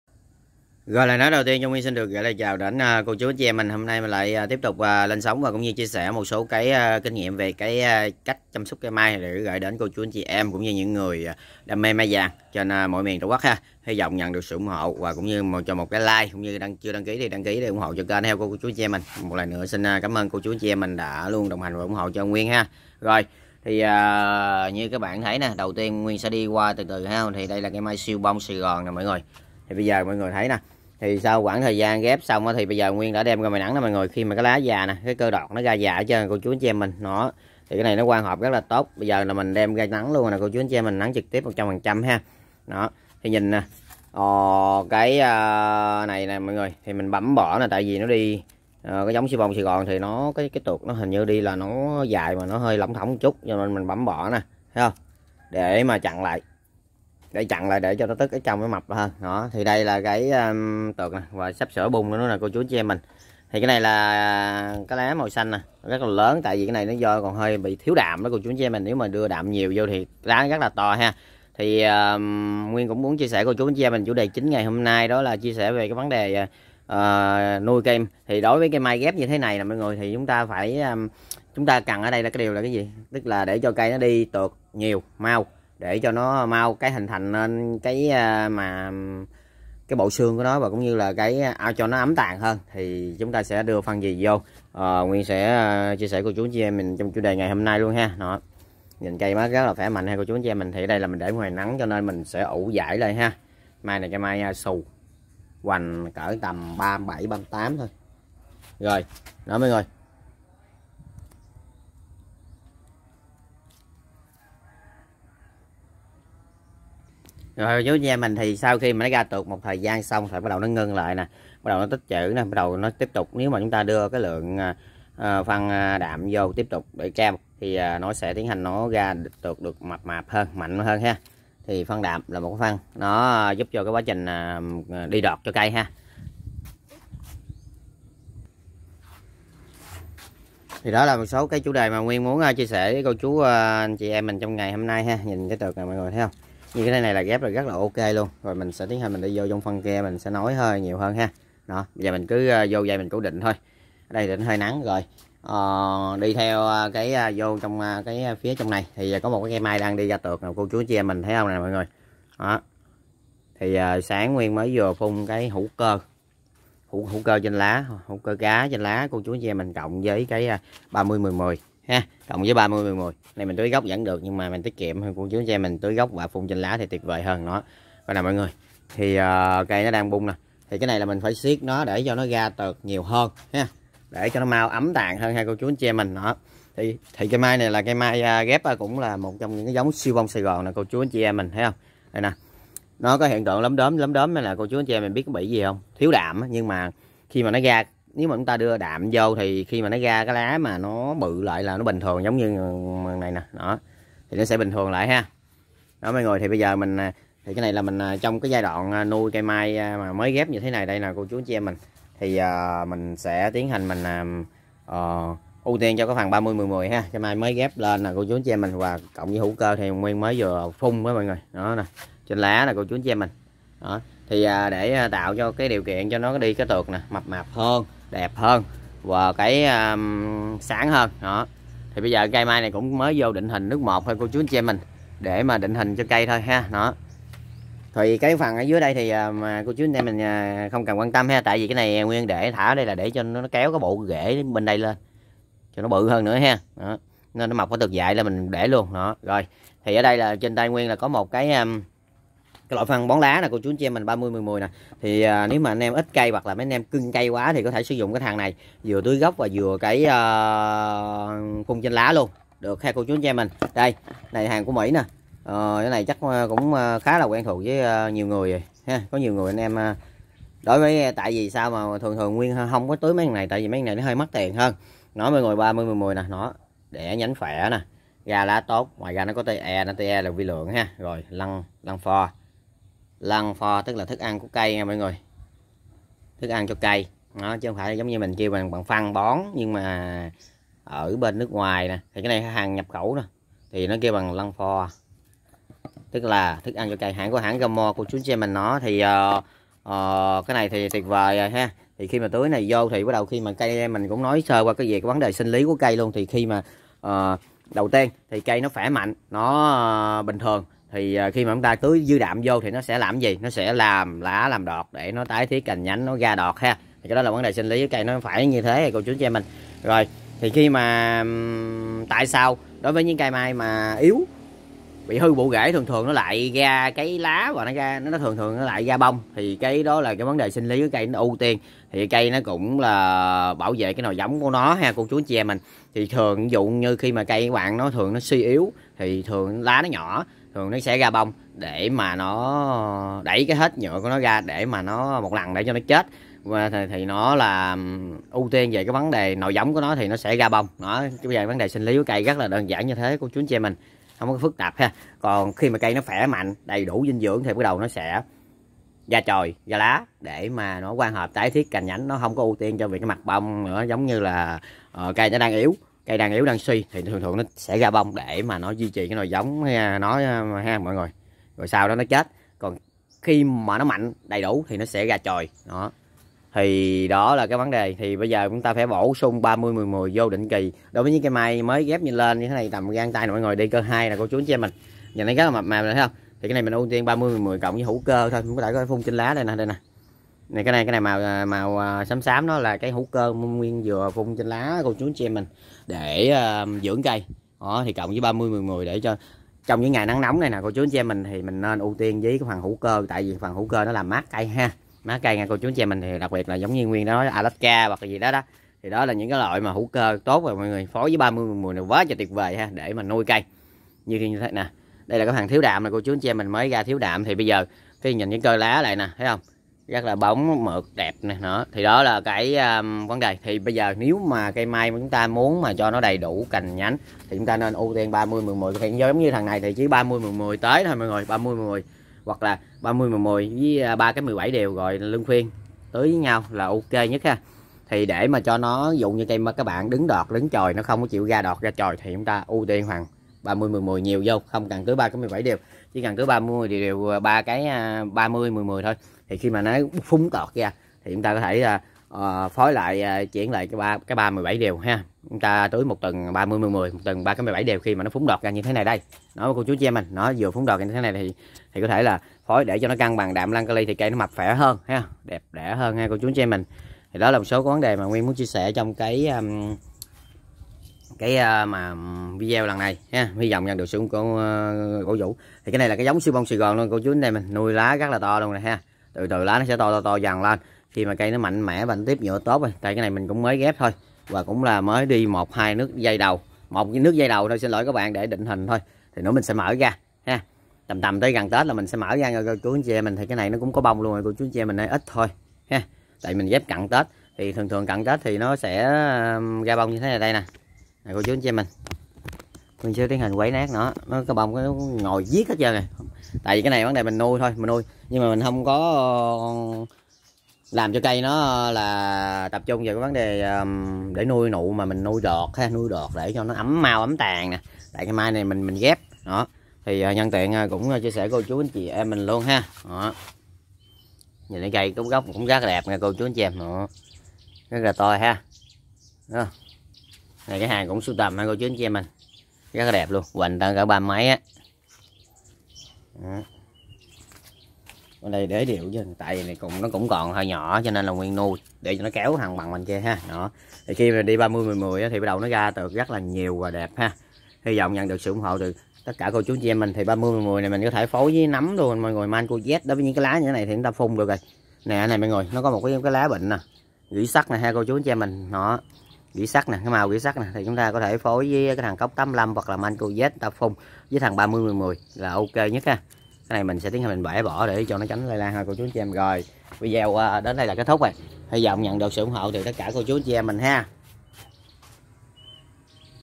Tưới phân npk nào cho cây mai ra đọt. Kinh nghiệm trồng mai
Tiếp chia sẽ kinh nghiẽm trồng mai vàng sau khi ghép bằng cách bón thúc giúp cây đi tược mạnh mau kính tàng chơi bonsai